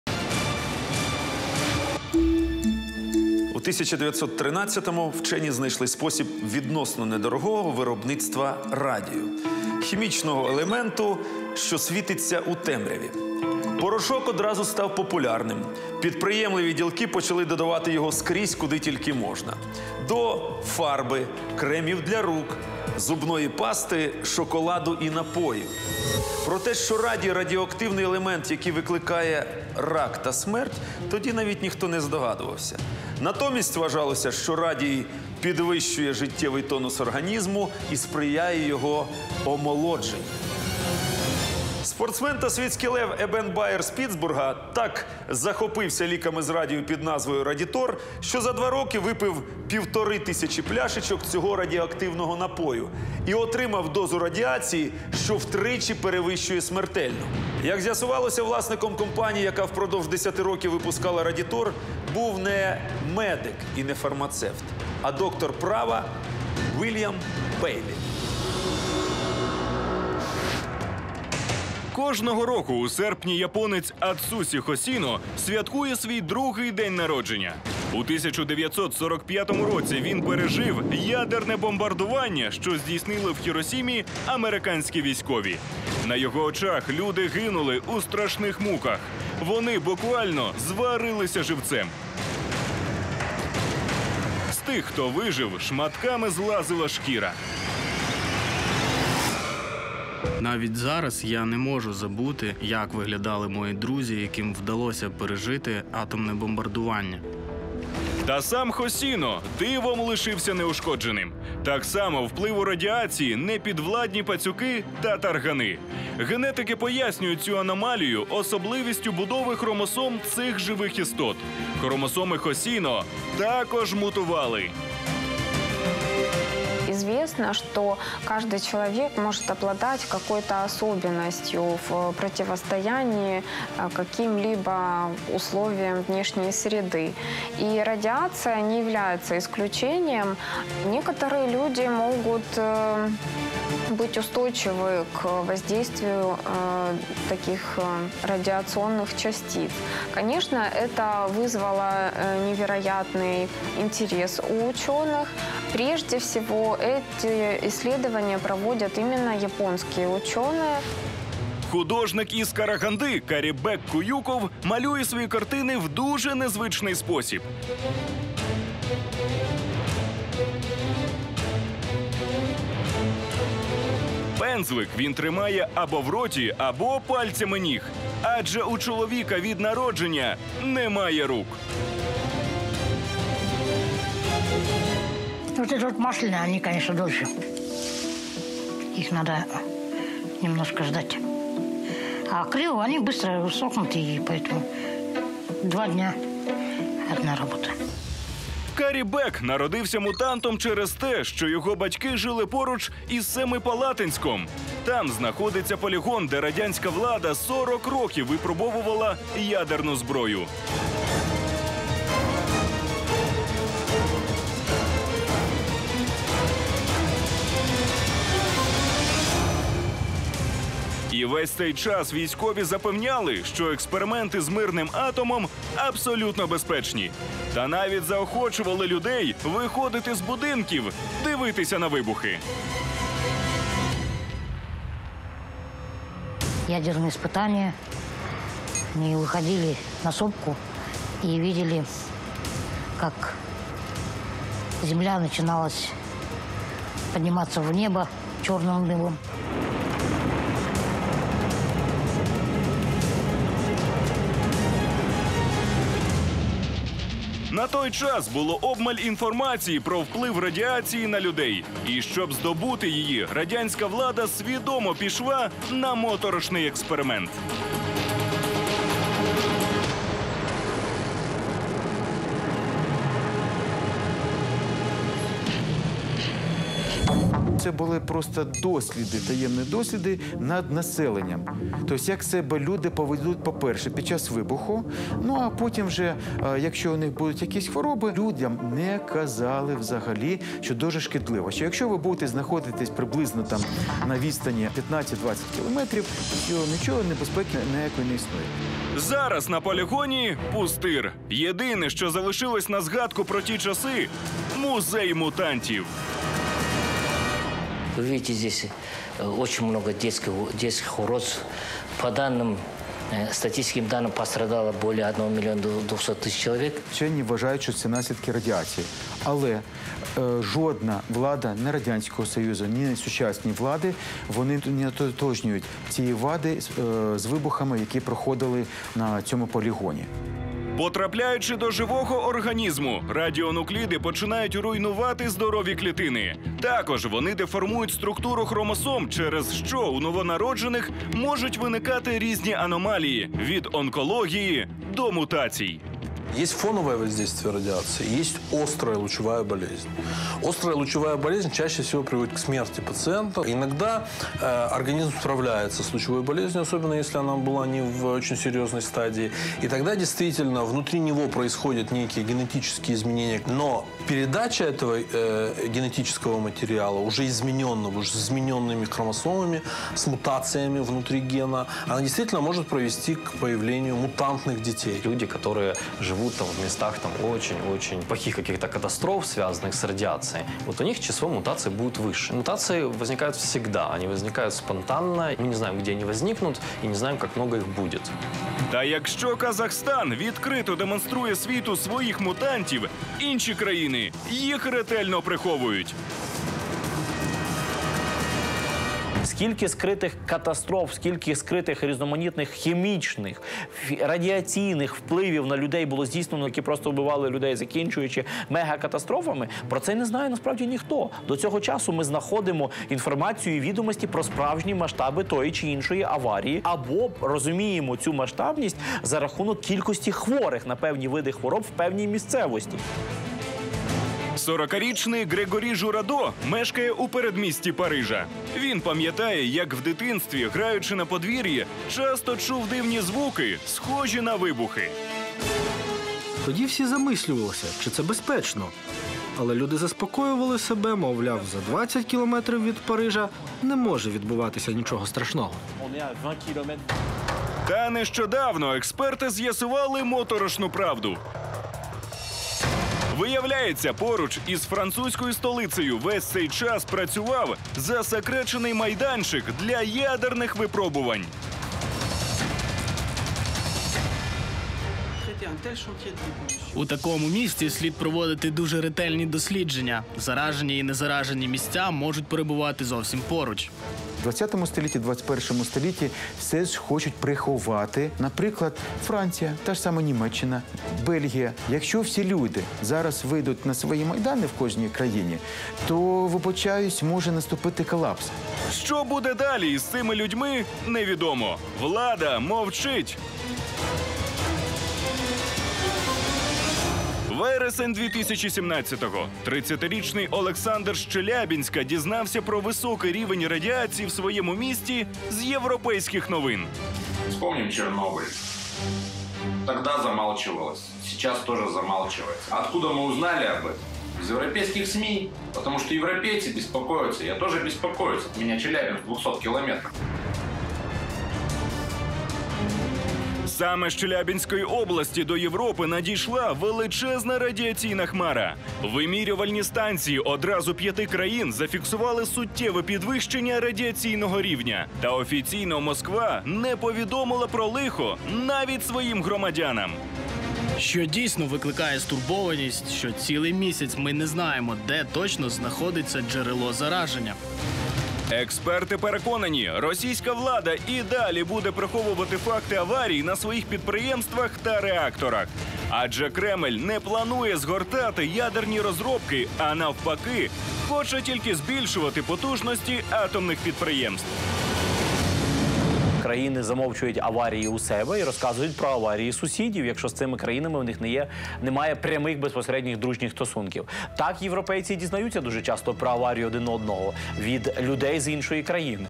Радіоактивні мутанти
Радіація вбиває, але не всіх. Радіація не діє на щурів та тарганів, а також на деяких людей. Як радіонукліди впливають на людину та чому уряди різних країн замовчують проблеми із ядерними випробуваннями?